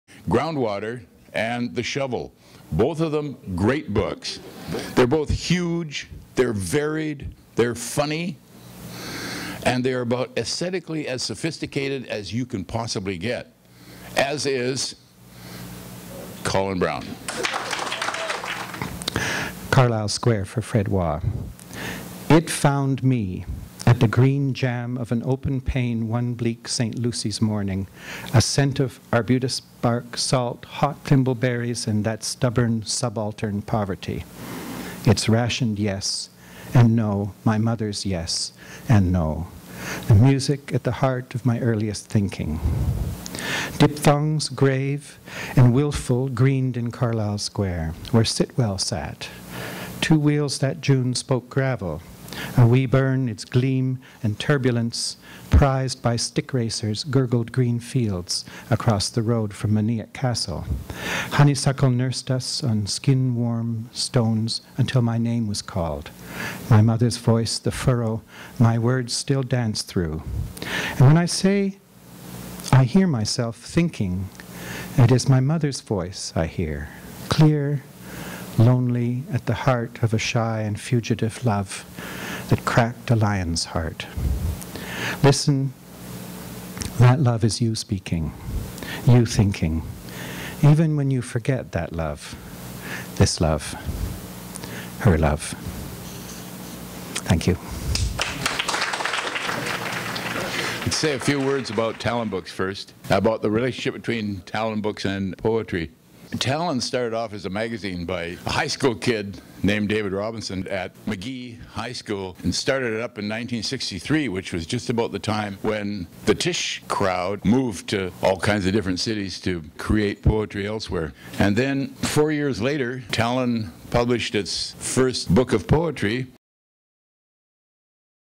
Colin Browne - Fred Wah and Friends Poetry Reading; Vancouver, BC Canada (part 2 of 7)
Talonbooks Poetry Night: Fred Wah and Friends. Colin Browne is a writer, filmmaker, teacher, critic and poet. Biography | Event features poetry from: George Bowering, Colin Browne, Jeff Derksen, Daphne Marlatt (reading from her Canadian Japanese Noh play 'The Gull'), Roy Miki, and Fred Wah. Event date: September 30, 2009 at the Anza Club in Vancouver, BC.